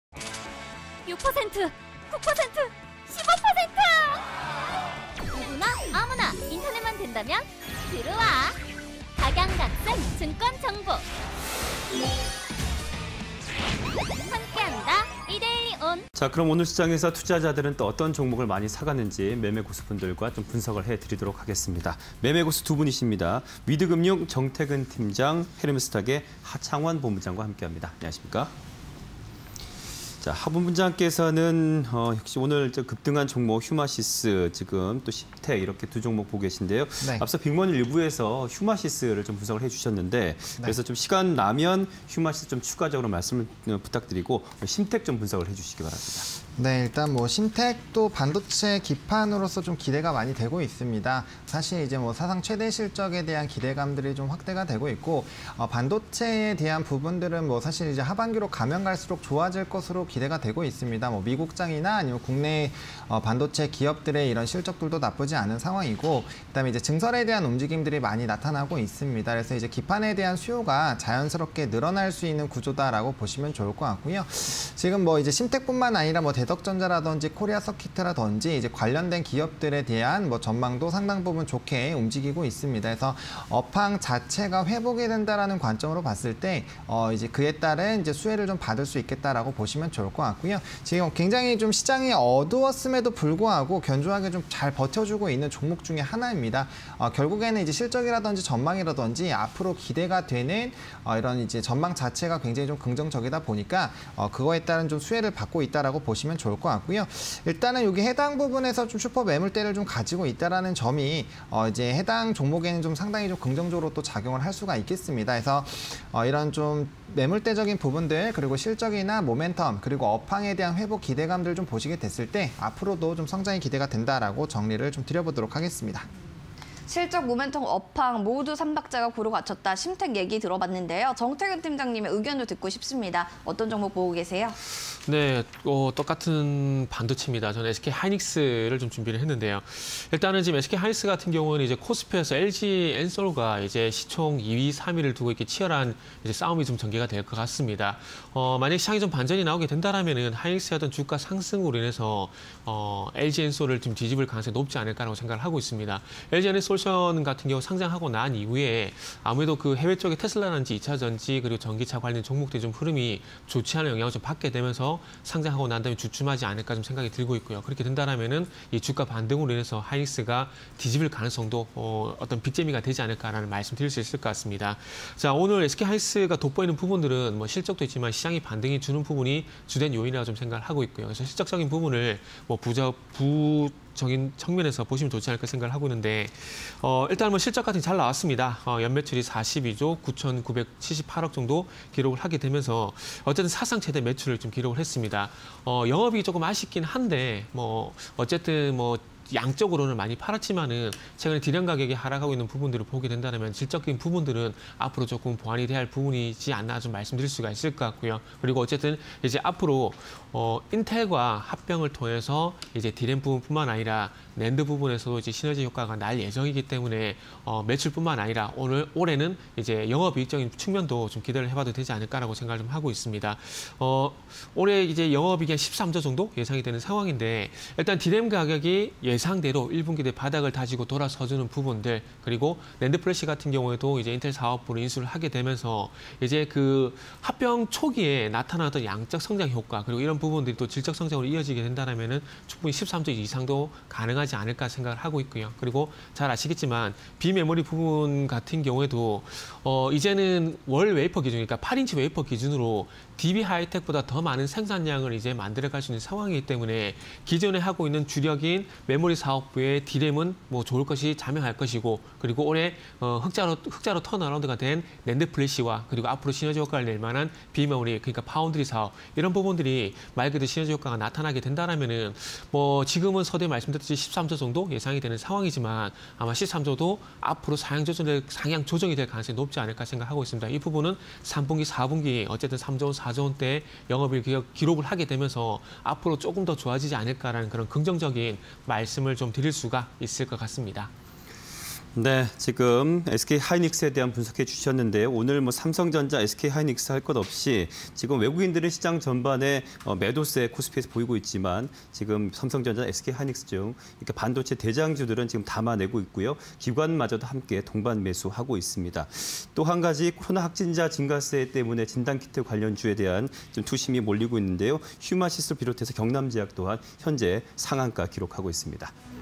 심텍(222800), 휴마시스(205470)_사가지 (20220128)
심텍(222800)

- 연매출 42조 9,978억… 사상 최대 기록
- DRAM 수요 증가ㆍNAND 플래시 흑자전환
- 인텔과 NANA사업 시너지ㆍ비메모리 확장 기대